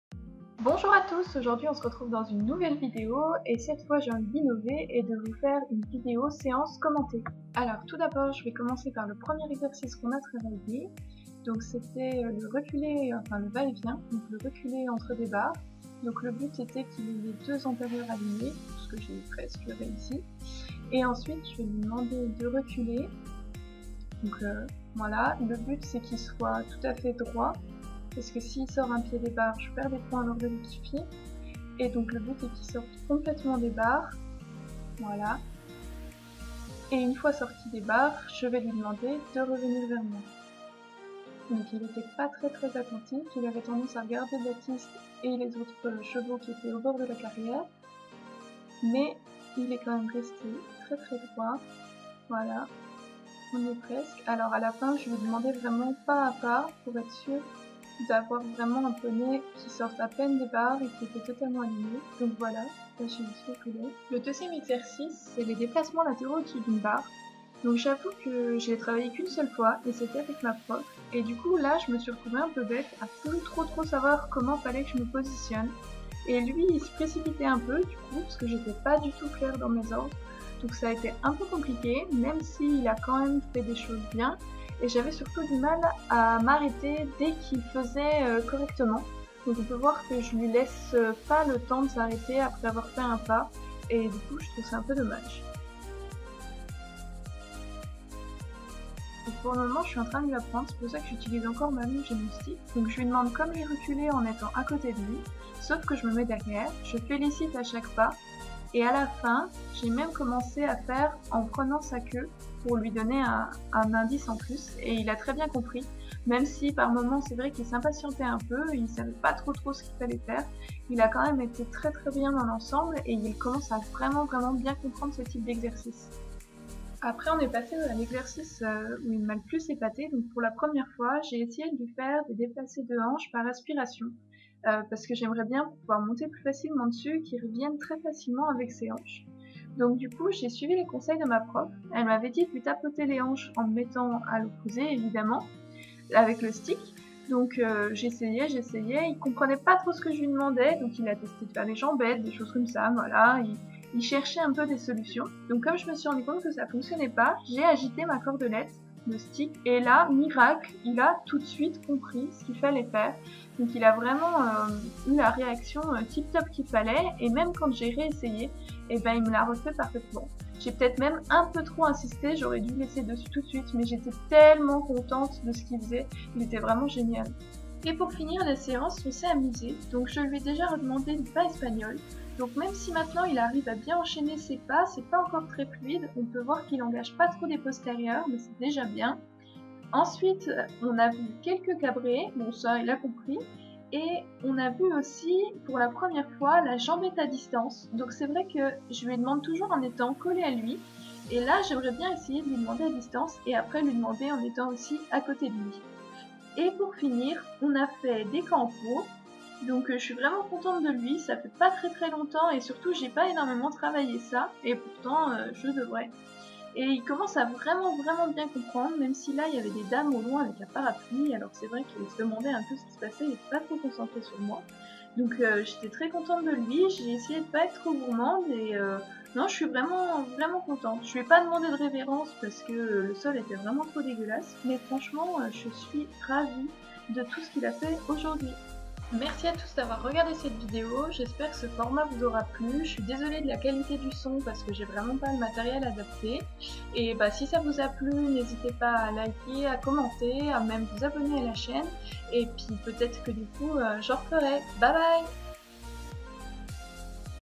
[SEANCE COMMENTEE] Travail à pied le 04 mars 2018
Hello!
J'ai voulu tenter un nouveau format aujourd'hui avec une vidéo de séance commentée, j'espère que ça vous plaira.

Matériel utilisé :

Lightworks